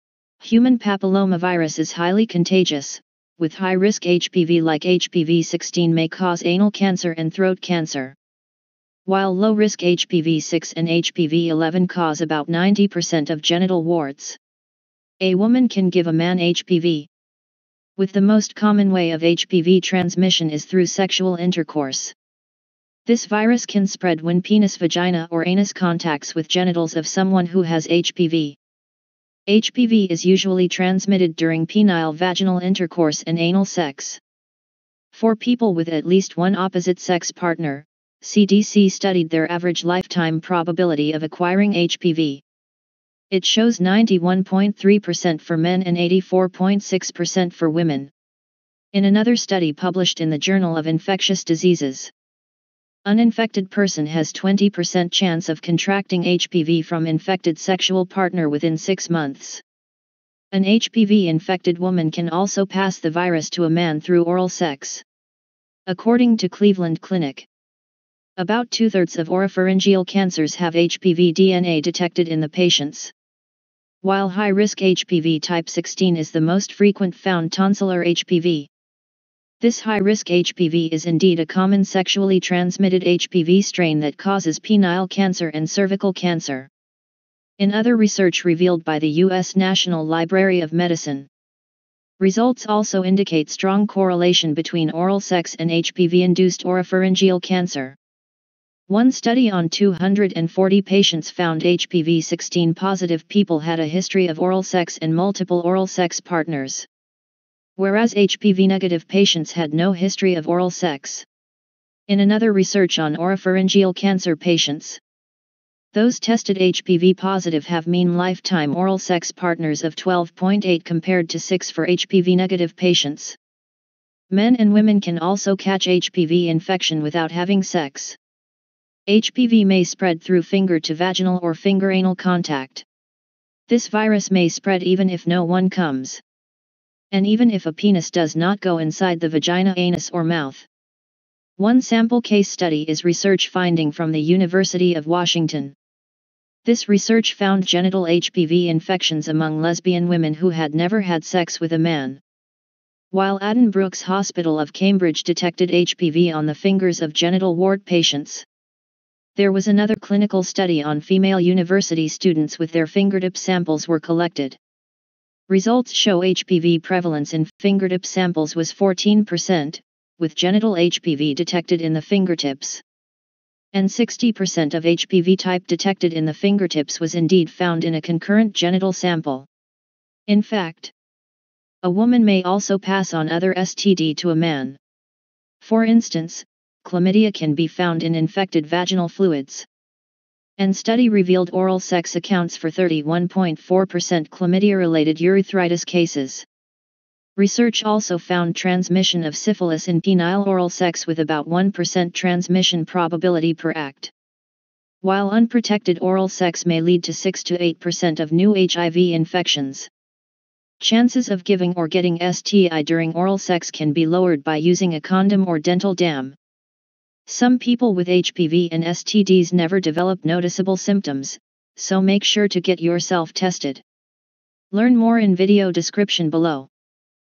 Can a Woman Give a Man HPV
Can a woman give a man HPV and STD?

Human Papillomavirus is highly contagious and can easily spread, with high risk HPV like HPV-16 may cause anal cancer and throat cancer, while low risk HPV-6 and HPV-11 cause about 90% of genital warts in men and women.

This video address the common question of whether man can get HPV and STD from woman, including research and HPV transmission studies published by the U.S. CDC, the Journal of Infectious Diseases, National Library of Medicine, University of Washington, etc.

Watch the video to learn more on how is HPV transmitted, and whether you can get HPV without sex.

Some people with HPV and STDs never develop noticeable symptoms, so make sure to get yourself tested.